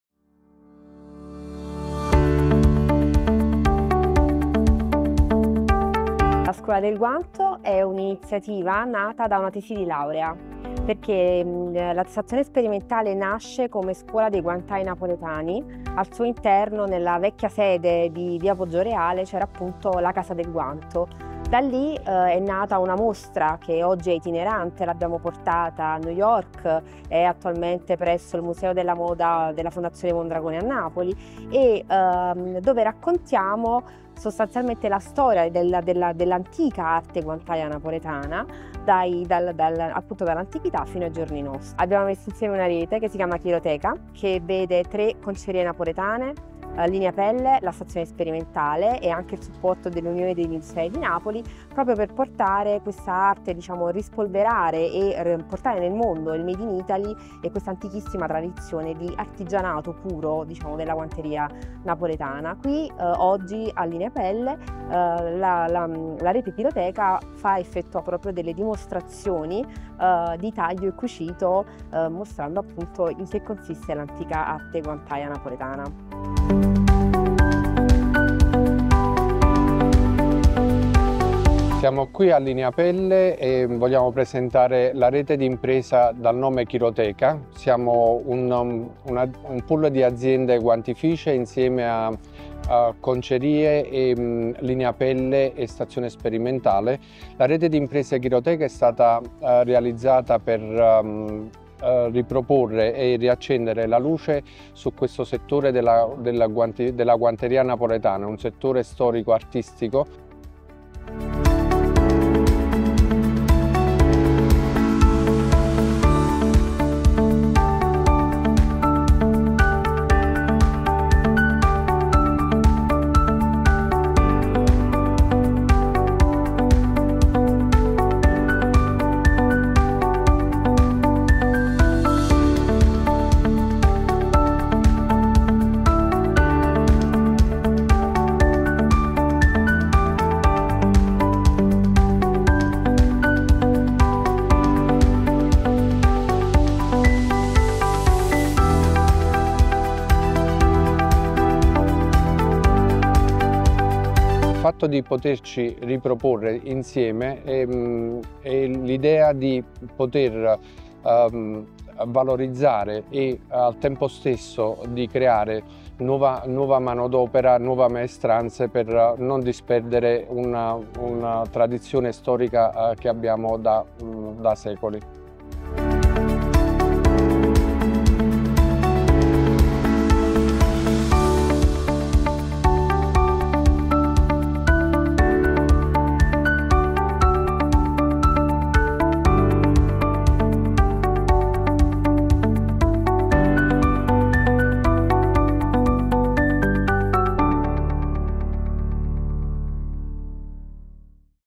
Un omaggio alla tradizione del guanto in pelle - Lineapelle 2024
Grande protagonista della fiera è stata “La Scuola del Guanto”, la mostra in collaborazione con Chiroteca Business Network, Lineapelle e patrocinata dalla Regione Campania, il cui scopo è promuovere e tutelare quest’antica e nobile arte.
A great protagonist of the fair was 'The Glove School’, the exhibition in collaboration with Chiroteca Business Network, Lineapelle and sponsored by the Campania Region, whose aim is to promote and protect this ancient and noble art.